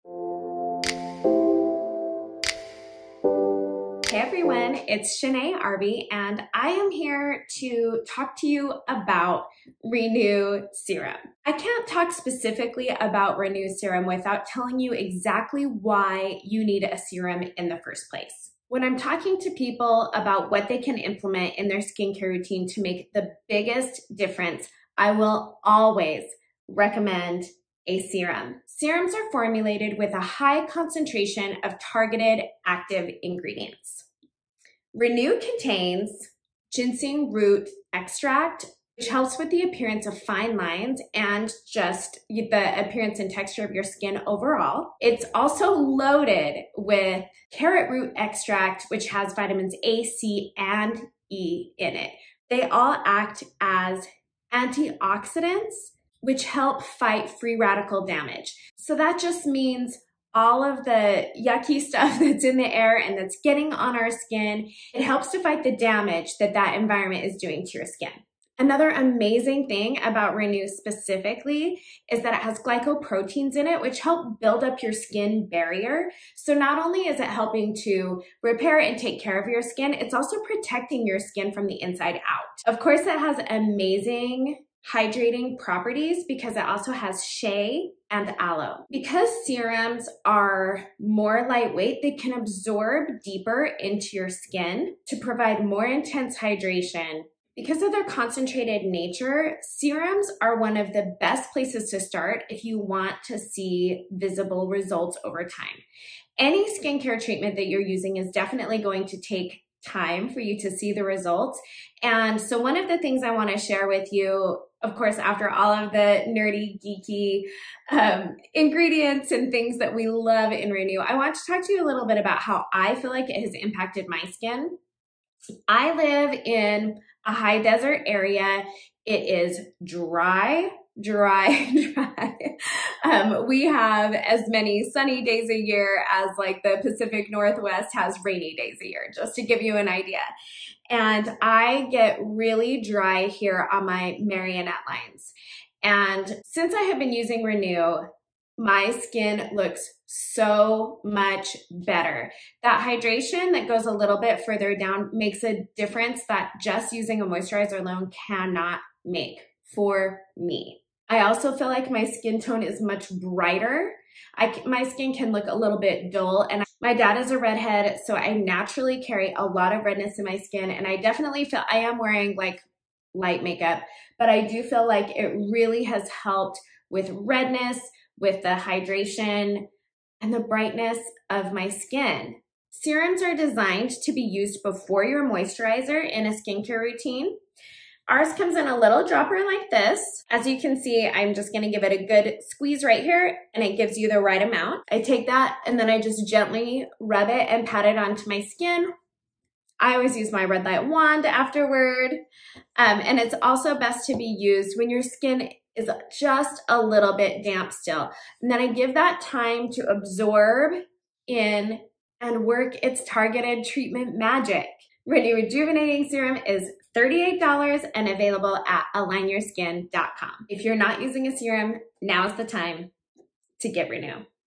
Review: Renew Rejuvenating Serum
Renew is a Rejuvenating Daily Serum. Serums are light, easily absorbed liquids for skin with higher concentrates of unique ingredients for a specific purpose. Renew features ingredients that target fine lines and the signs of aging as well as improve skin texture. Renew may boost hydration, improve skin tone, and improve skin texture. Renew was formulated to be used alone or in conjunction with the ALIGN Red Light wand.